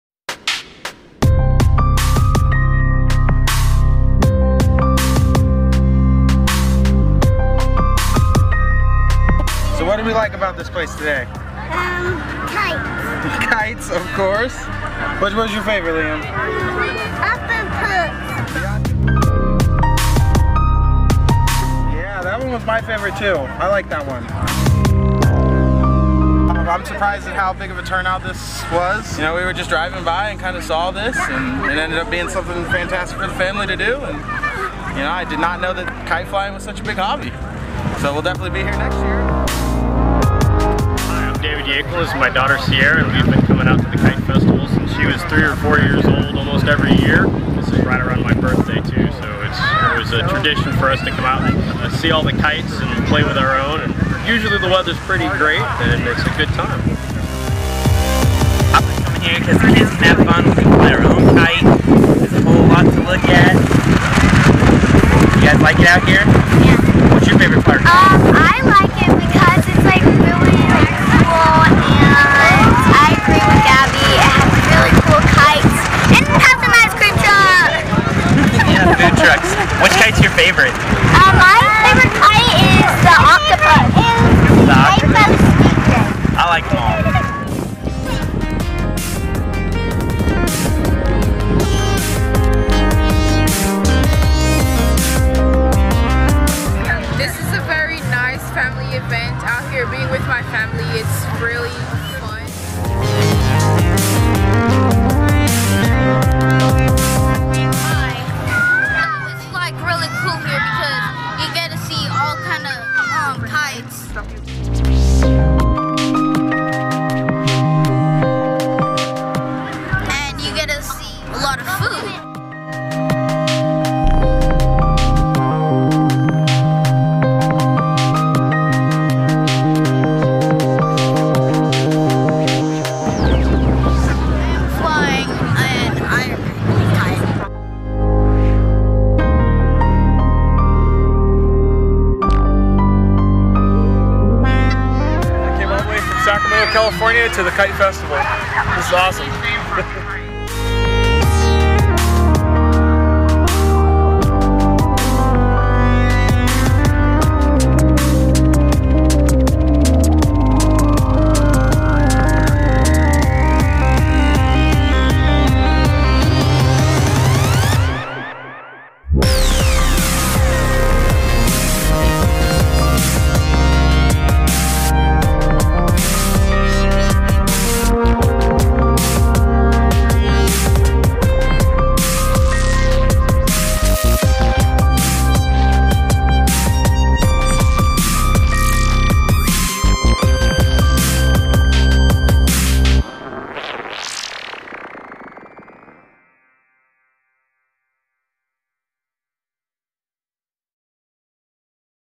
Flights of Fancy Kite Festival 2018
Colorful kites decorate the skies and over 15 thousand people in attendance that day.
Giant kite show by Great American Kites and Events.